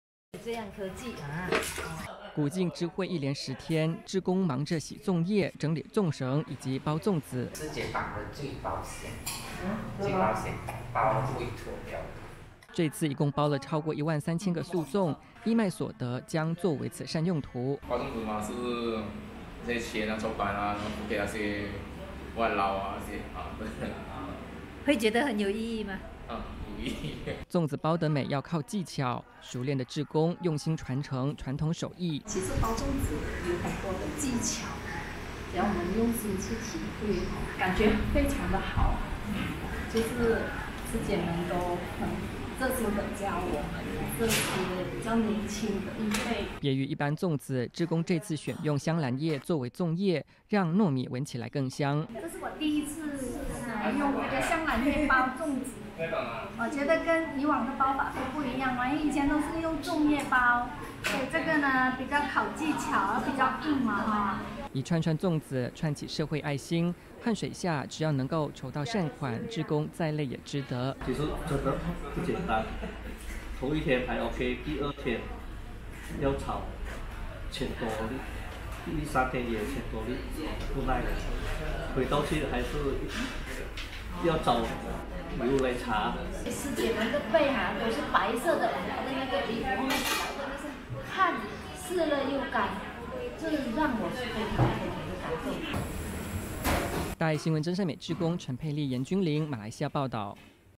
萬粽一心 善愛相繫
馬來西亞古晉慈濟志工製作素粽義賣，超過1萬3千個粽子，希望串連社會愛心做善事。